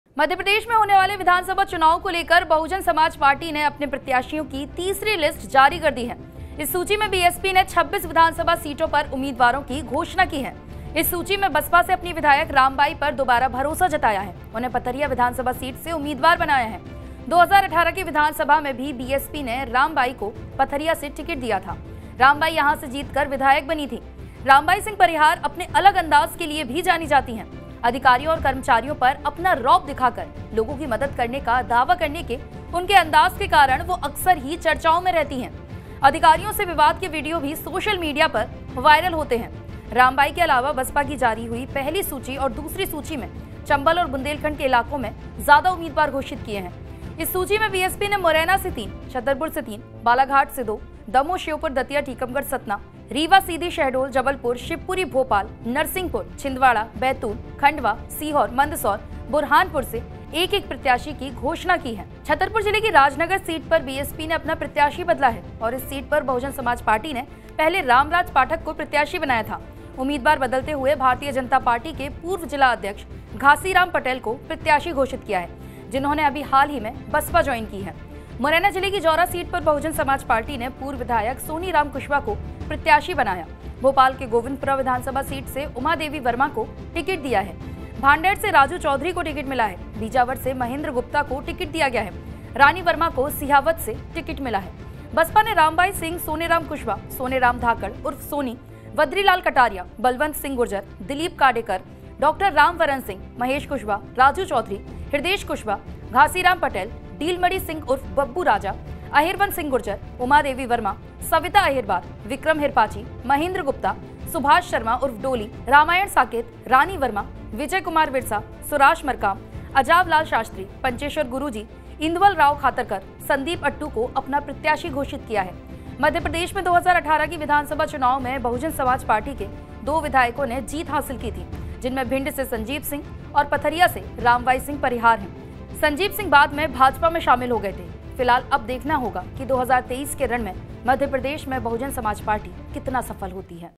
MP Election 2023: BSP ने जारी की अपनी तीसरी List, एकलौती विधायक Rambai singh parihar भी शामिल
Bahujan Samaj Party has announced the third list of its 26 assembly constituencies regarding the assembly elections to be held in Madhya Pradesh. The trusted supporters of second time MLA Rambai have made him a candidate from Patharia assembly seat...In the 2018 assembly also BSP had given ticket to Rambai from Patharia...Rambai was a trusted MLA from here...see who else is included in the list of MLAs?

मप्र में होने वाले विधानसभा चुनाव को लेकर बहुजन समाज पार्टी ने अपने प्रत्याशियों की तीसरी लिस्ट की जारी कर दी है .इस सूची में बीएसपी ने 26 विधानसभा सीटों पर उम्मीदवारों की घोषणा की है. इस सूची में बसपा ने अपनी विधायक राम बाई पर दौबारा भरोसा जताया है उन्हें पथरिया विधानसभा सीट से उमीदवार बनाया है ...2018 के विधानसभा में भी बीएसपी ने रामबाई को पथरिया से टिकट दिया था. रामबाई यहां से जीतकर विधायक बनी थी... देखिए और किस-किस के नाम शामिल